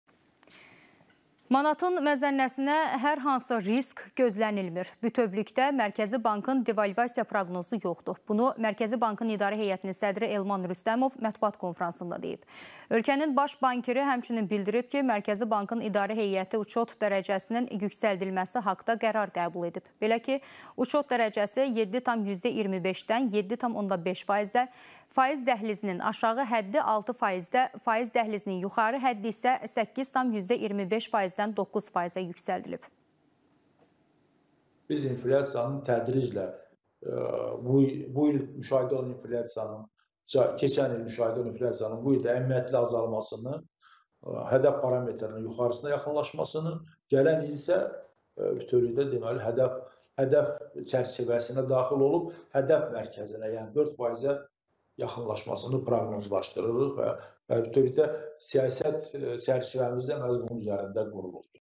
Elman Rüstəmov: "Bu il manatın məzənnəsi stabil qalacaq, devalvasiya olmayacaq"
Manatın məzənnəsinə hər hansı risk gözlənilmir. Bütövlükdə Mərkəzi Bankın devalvasiya proqnozu yoxdur. Bunu Mərkəzi Bankın İdarə Heyətinin sədri Elman Rüstəmov mətbuat konfransında deyib. Ölkənin baş bankiri həmçinin bildirib ki, Mərkəzi Bankın İdarə Heyəti uçot dərəcəsinin yüksəldilməsi haqda qərar qəbul edib. Belə ki, uçot dərəcəsi 7,25-dən 7,5%-ə, faiz dəhlizinin aşağı həddi 6%-də, faiz dəhlizinin yuxarı həddi isə 8,25%-dən 9%-ə yüksəldilib.